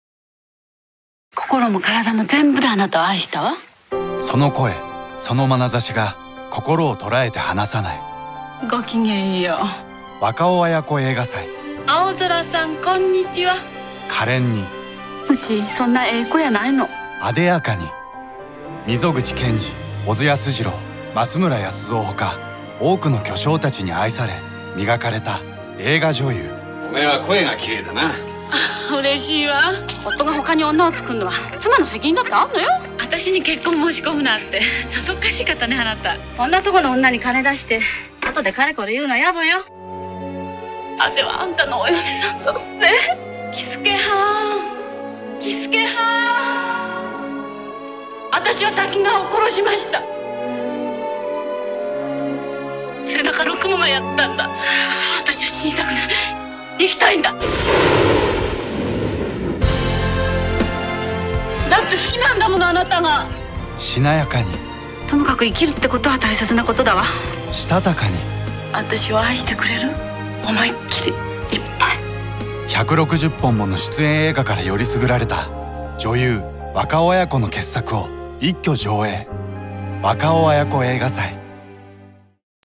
「若尾文子映画祭」予告編
「若尾文子映画祭」が2020年2月28日より角川シネマ有楽町を皮切りに、全国順次開催されることが決定。あわせて予告編とビジュアルが公開された。

■公開情報
「若尾文子映画祭2020」
2020年2月28日（金）より角川シネマ有楽町ほか、大阪（シネヌーヴォ）、大分（シネマ 5）、福井（福井メトロ劇場）、石川（シネモンド）全国順次上映
(c)KADOKAWA